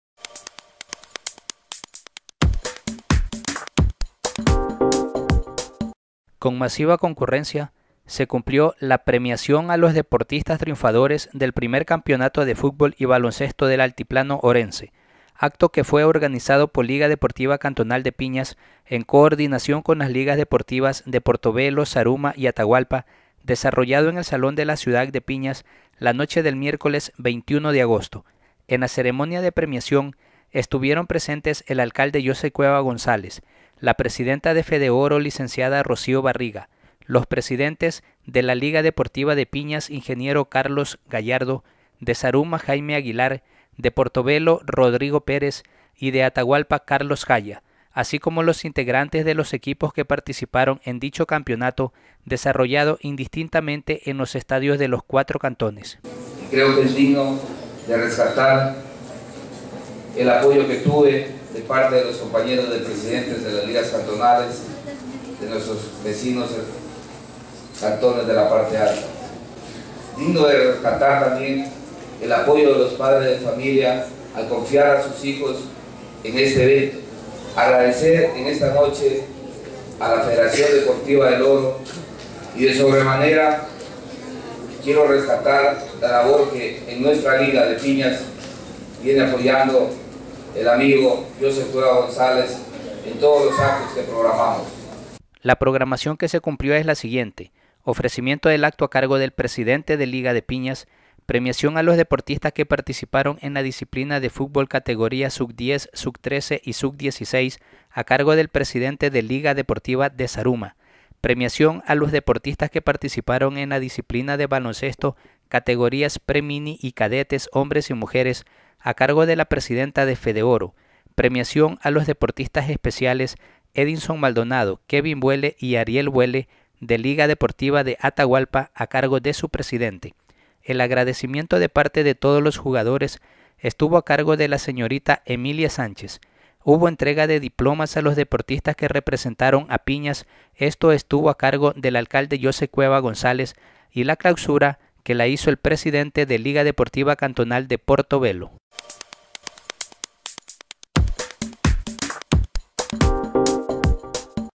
Premiación Primer Campeonato de Fútbol y Básquet del Altiplano
Con masiva concurrencia se cumplió la premiación a los deportistas triunfadores del Primer Campeonato de Fútbol y Baloncesto del Altiplano Orense, acto que fue organizado por Liga Deportiva Cantonal de Piñas en coordinación con las Ligas Deportivas de Portovelo, Zaruma y Atahualpa, desarrollado en el Salón de la Ciudad de Piñas la noche del miércoles anterior (21)